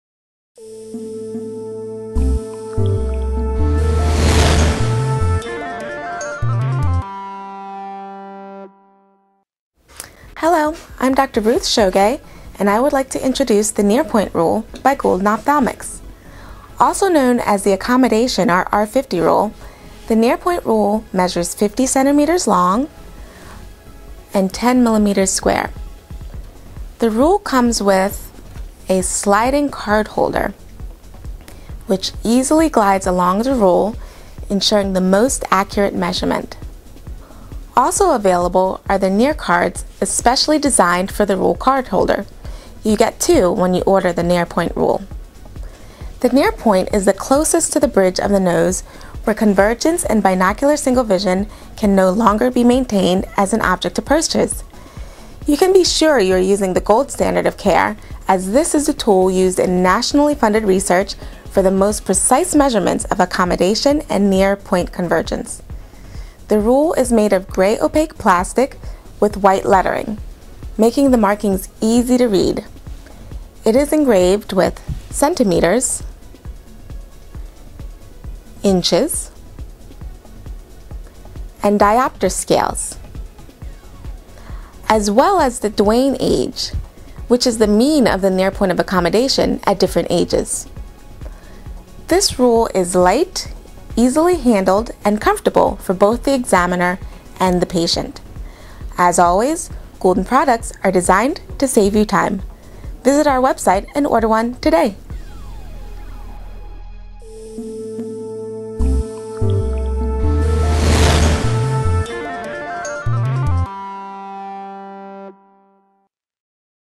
Near Point Rule
Near point is the point closest to the bridge of the nose where convergence and binocular single vision can no longer be maintained as an object approaches.

Also known as Accommodation and R-50 Rule, The Near Point Rule measures 50 cm long, 10 mm square, it is engraved with Centimeters, Inches, Diopter Scales and Duane Age (the mean of the near point of accommodation at different ages according to Duane) with sliding card holder.

Made of gray opaque plastic with white figures, the rules are light, easily handled, and are pleasant and agreeable for the examiner and patient to touch. The white lettering on gray make the markings easy to read.